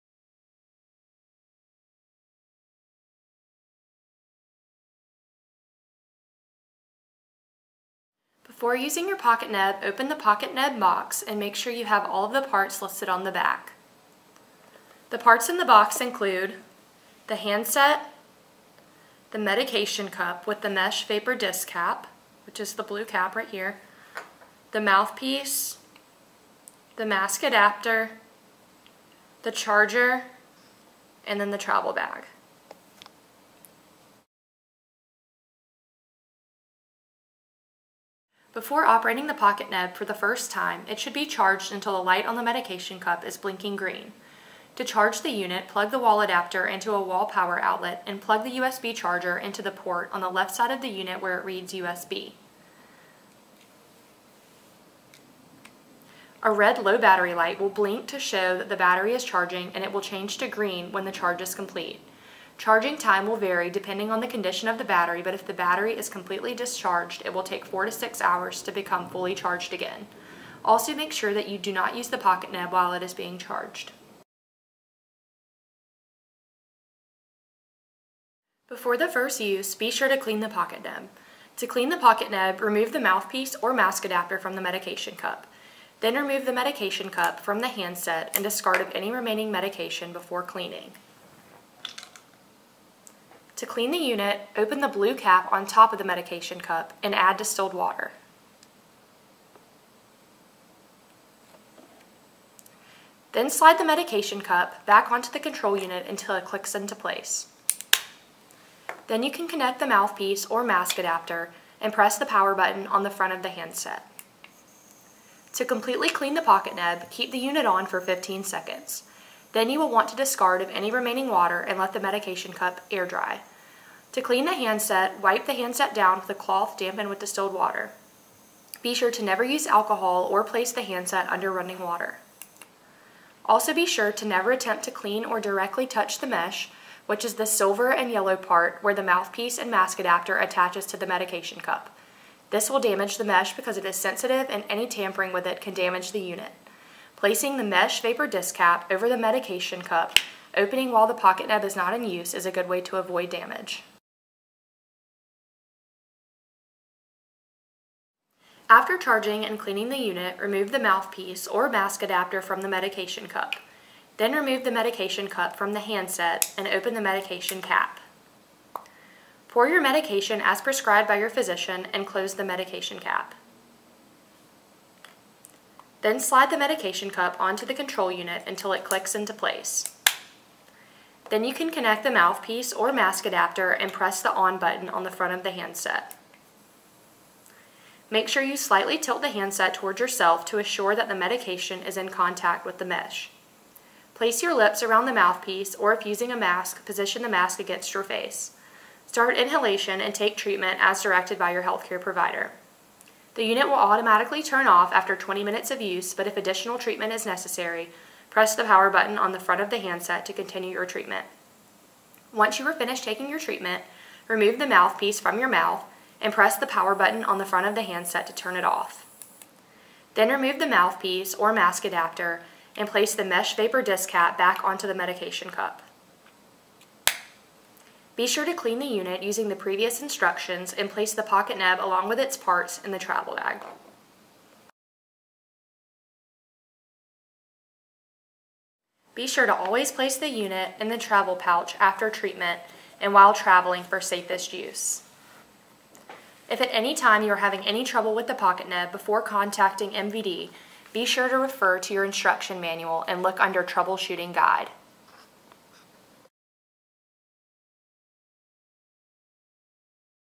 Micro Vapor Device || MVD-70
-Small compact design –weight 10 oz. with 6 ml medication capacity
-Lithium ion rechargeable battery - minimum 60 minutes of operation (90 minutes continuous)
-LED-indicator lights for power, charge, low battery
-Respirable aerosol: 1-5 microns
-Fast delivery (0.4 ml per minute average)
-Continuous or On-demand operation
-98% medication delivery
-Quiet operation